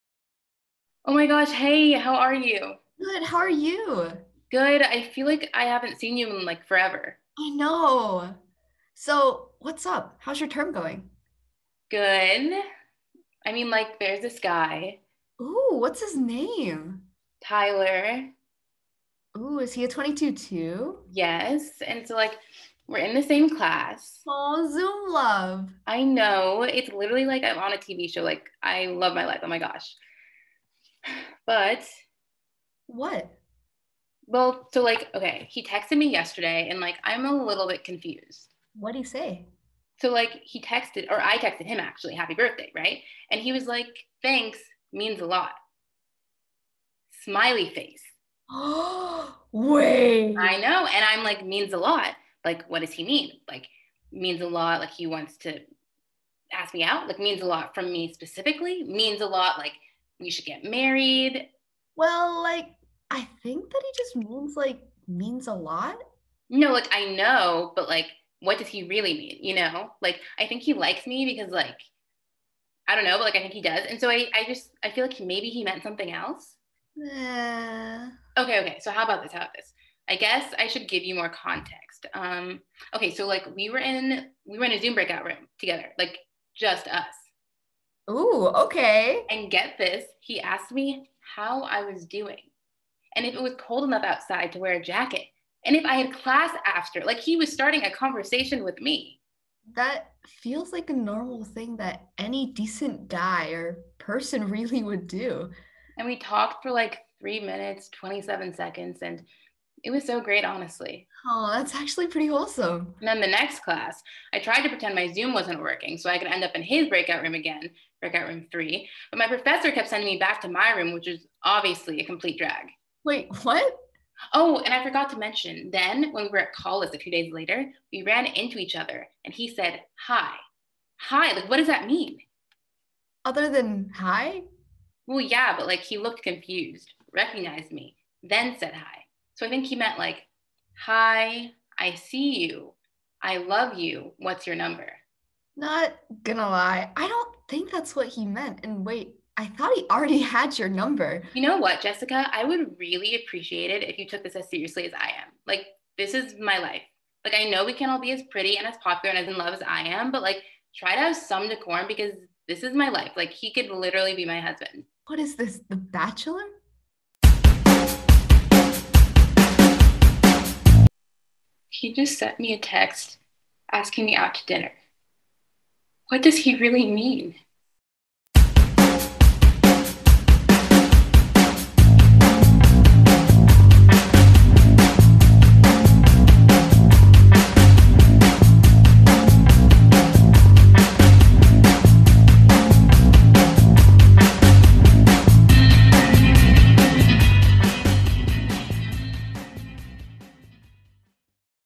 Okay, But What Does He Really Mean? - A Dominique Mobley sketch
In this sketch, written, directed, and edited by Dominique Mobley, a Girl Who Thinks The Bachelor Is Real Life (Dominique Mobley) ponders life's greatest mystery: "What Does He Really Mean?" Also starring Josephine Kim as Jessica.